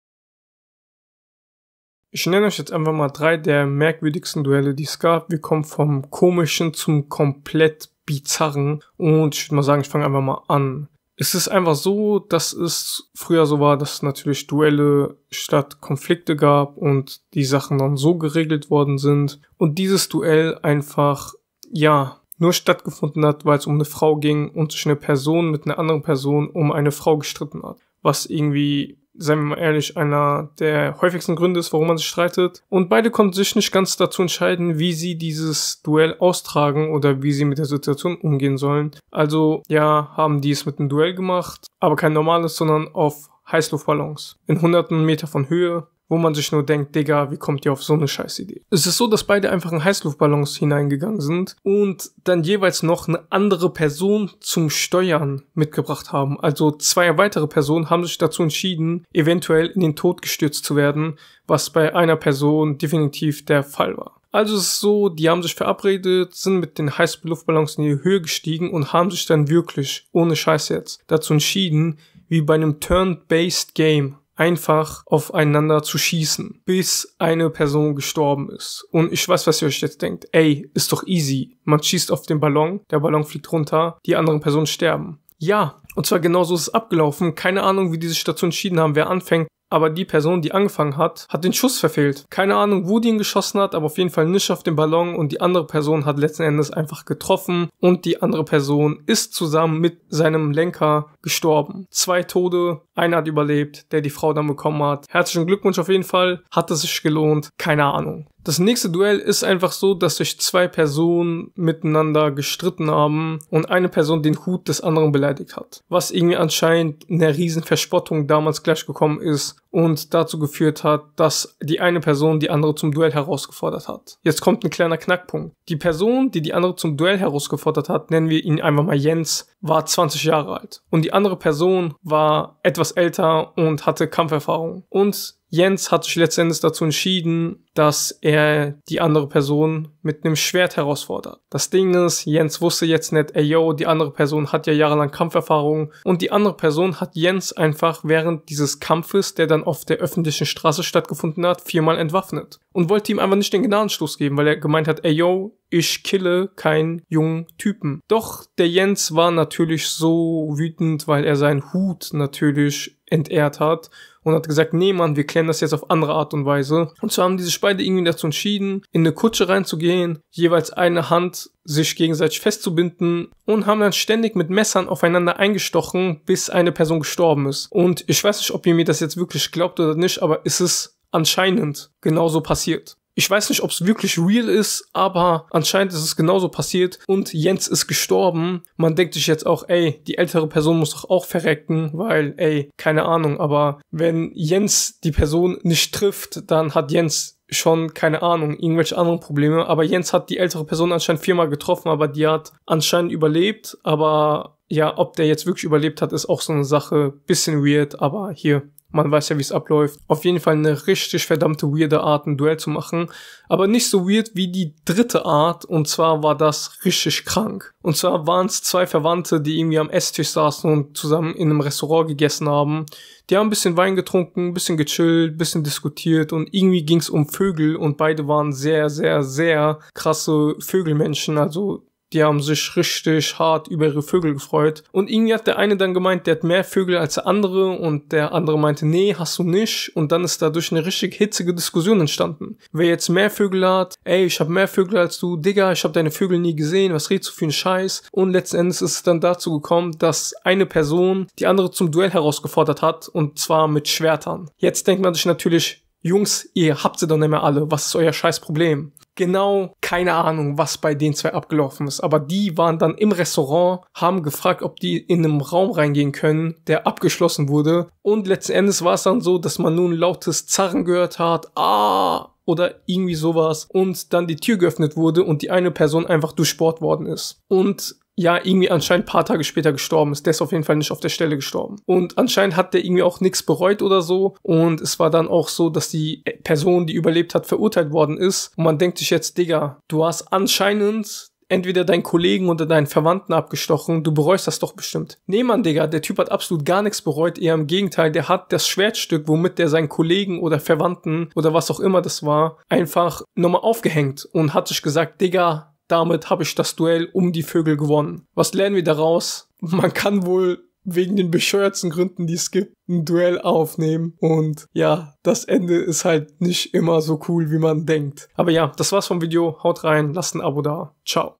Die Merkwürdigsten Duelle in der Geschichte
Sei es das sich wegen einem Hut duelliert wurde oder wegen einer Frau Duelle waren damals gang und geben und es ist nicht immer jemand dabei unverletzt herausgekommen  hier siehst du Beispiele für die merkwürdigsten Duelle und ihre gründe